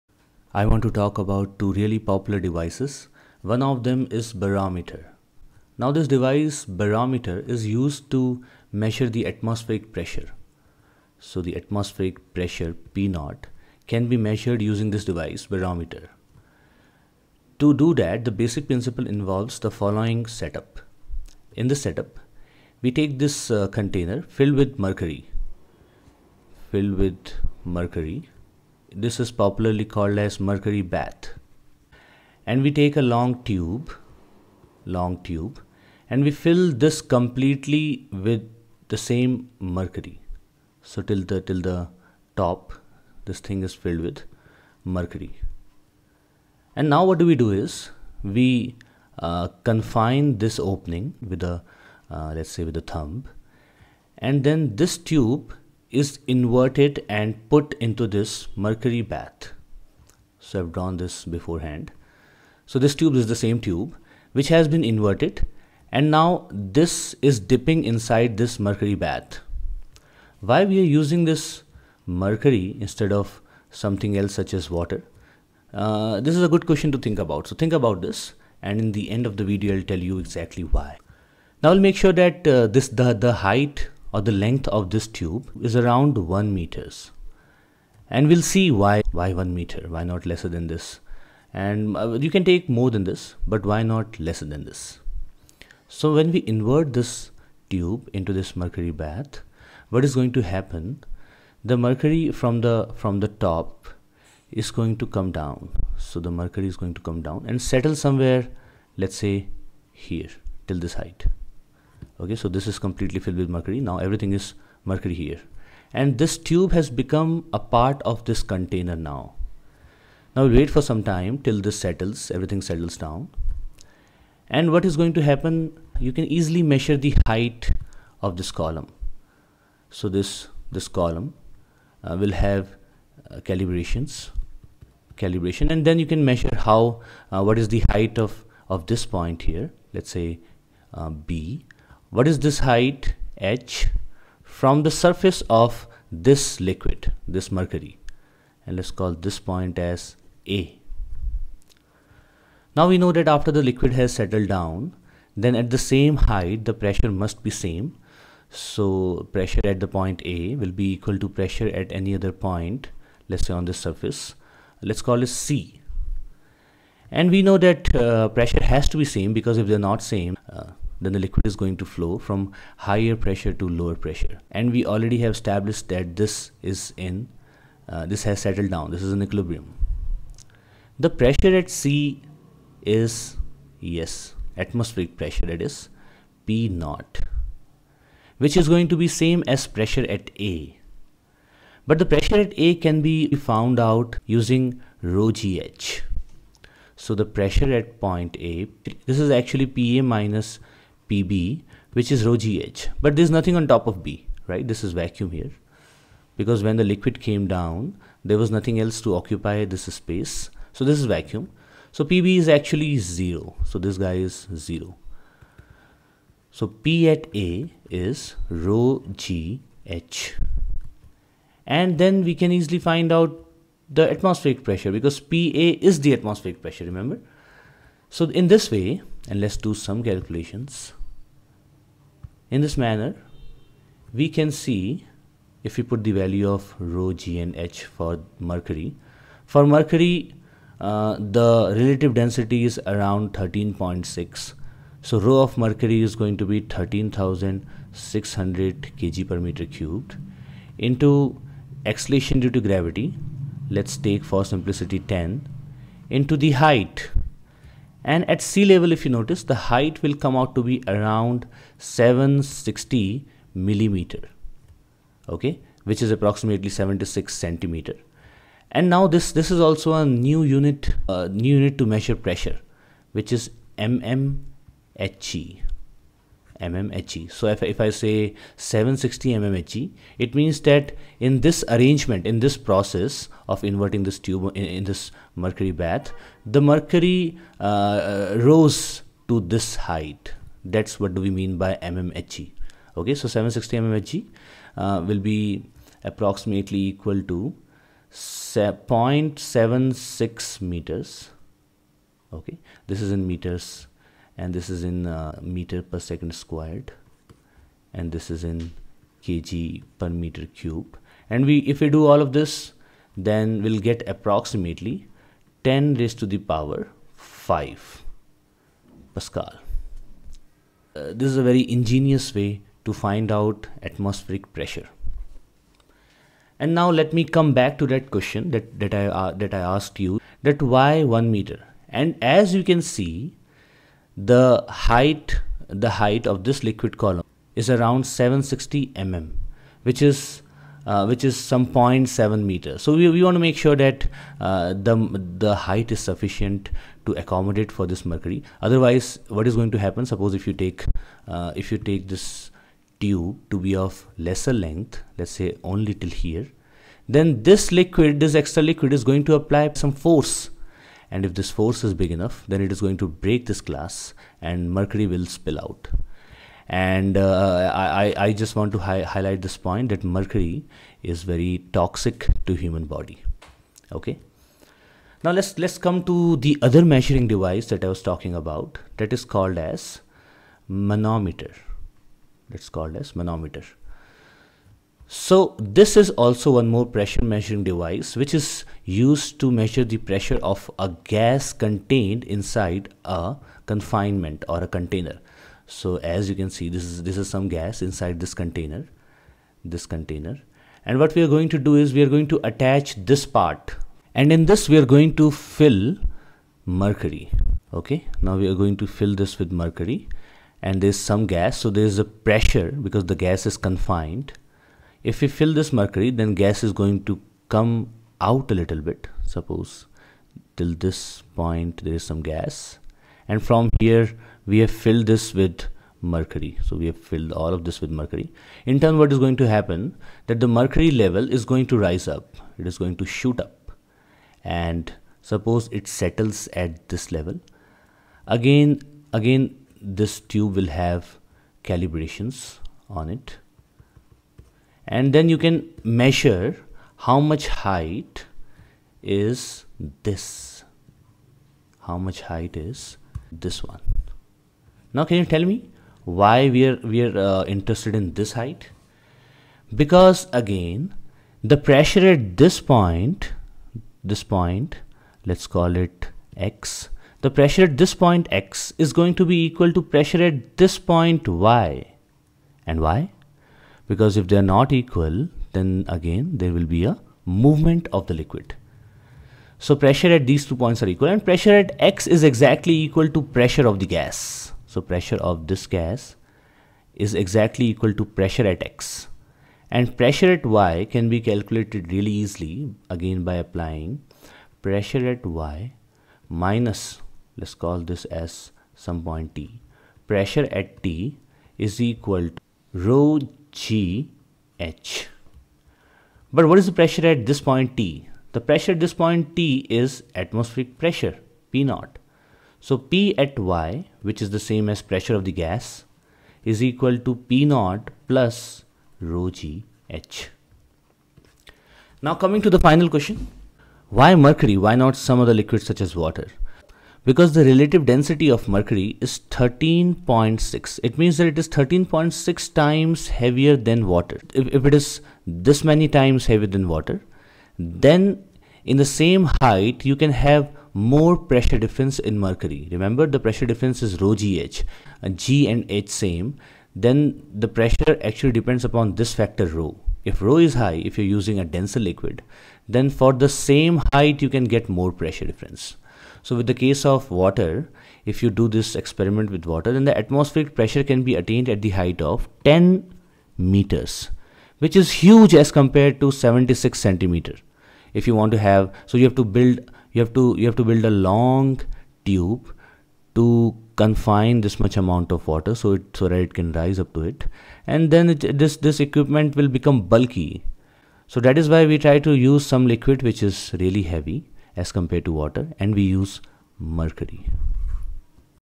Barometer & Manometer
Two important pressure measuring devices and the basic principle of their working.